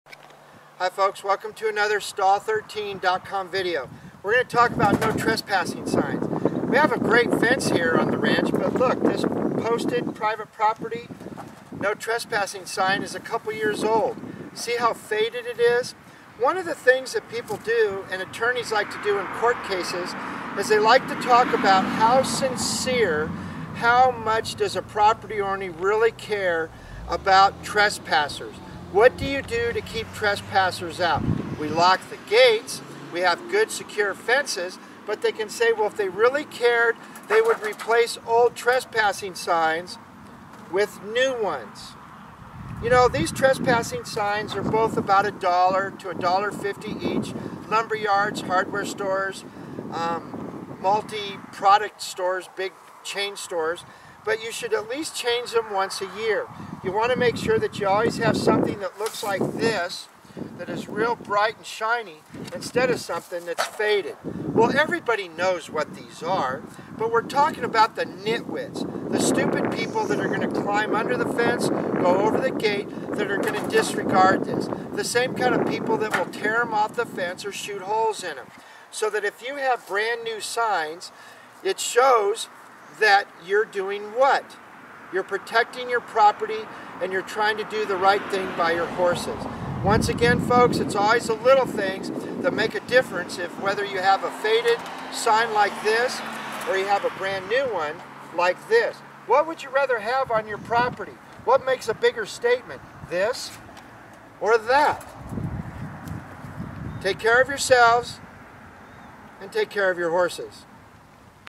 " NO TRESSPASSING " signs ! stall13.com videos
Every Horse barn and pasture should have a few posted !! To view more videos , to listen to an Internet radio show about Horses. " THE WORLD of HORSES "